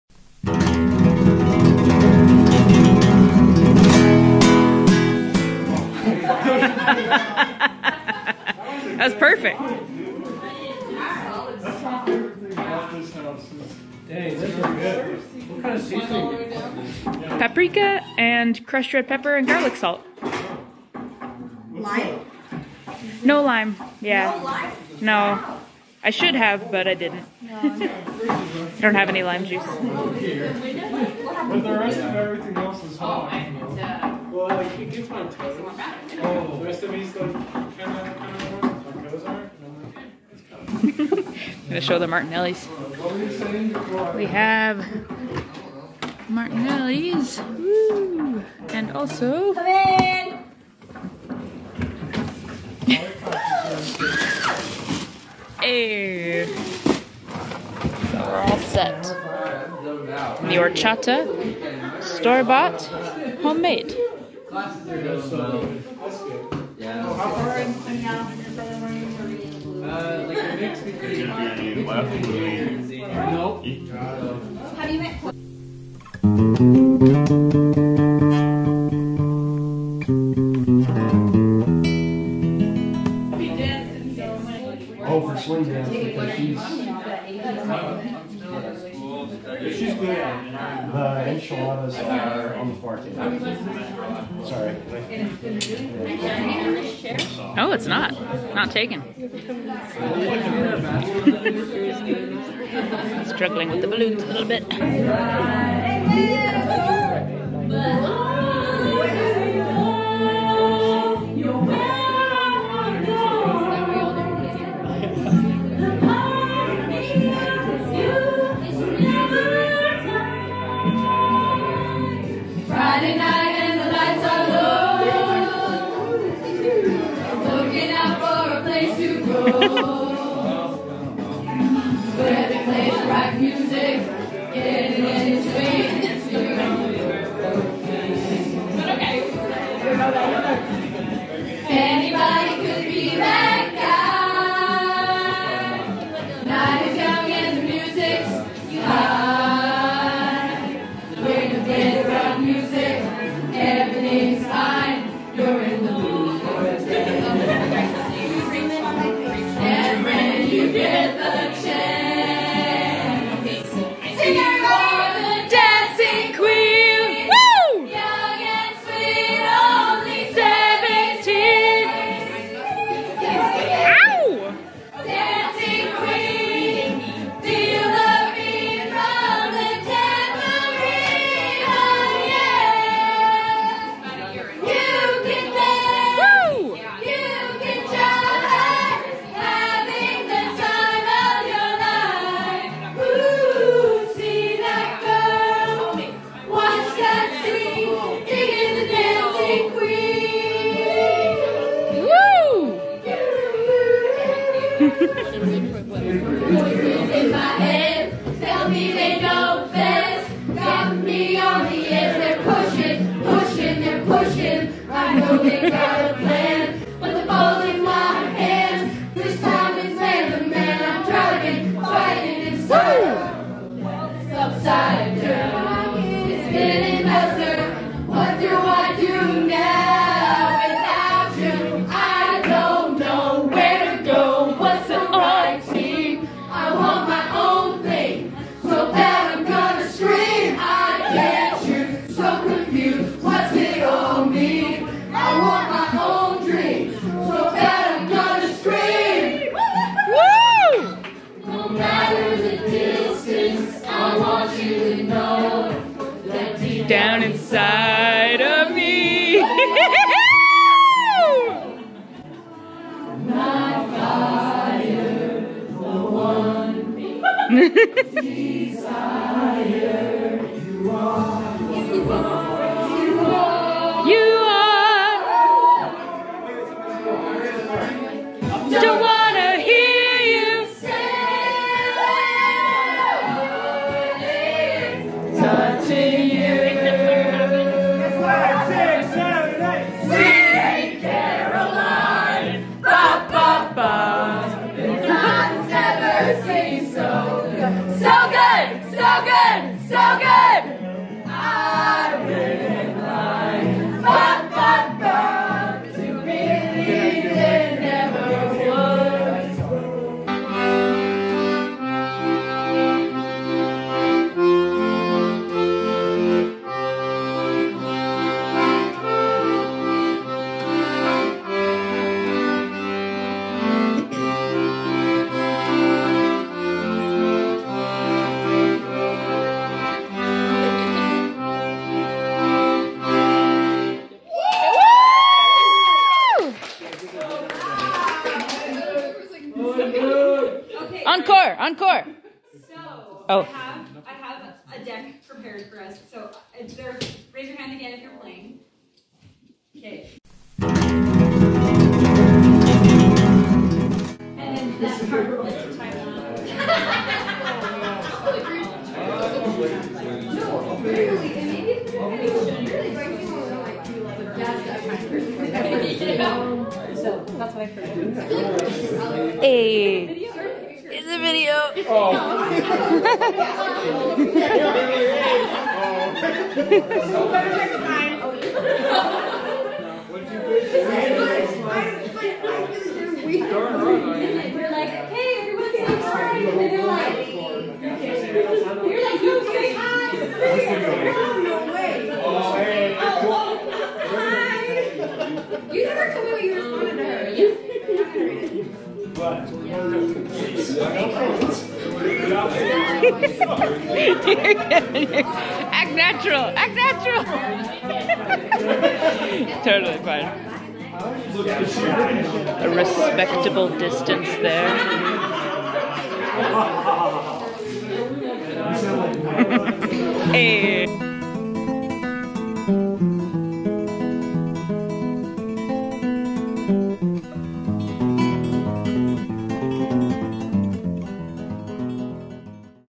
Anna's Birthday Party!
My friend Anna had her golden birthday - she turned 26 on the 26th! So I was happy to have her over with all our friends to help celebrate!!